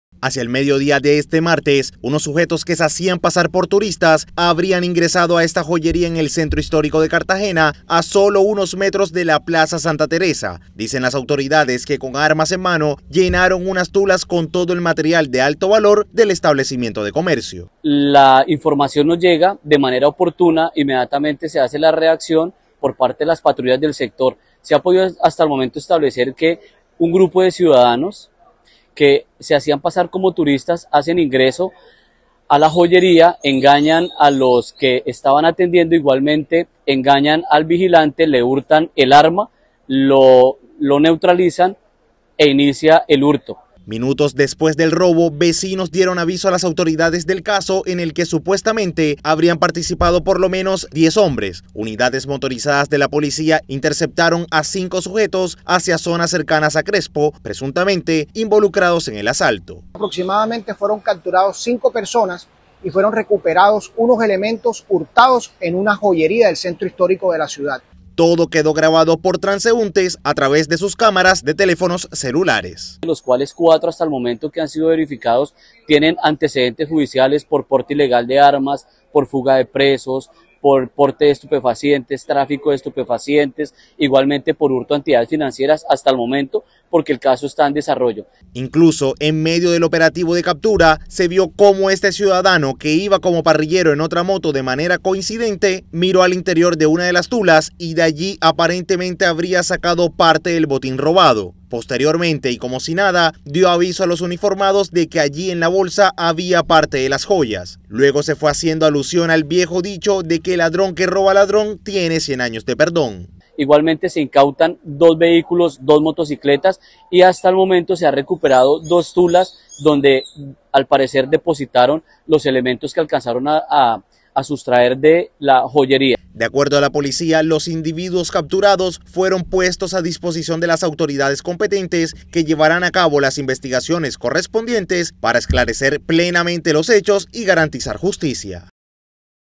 Así fue la captura de cinco presuntos ladrones de joyería en el centro histórico de Cartagena
CINCO CAPTURADOS TRAS ROBO A JOYERÍA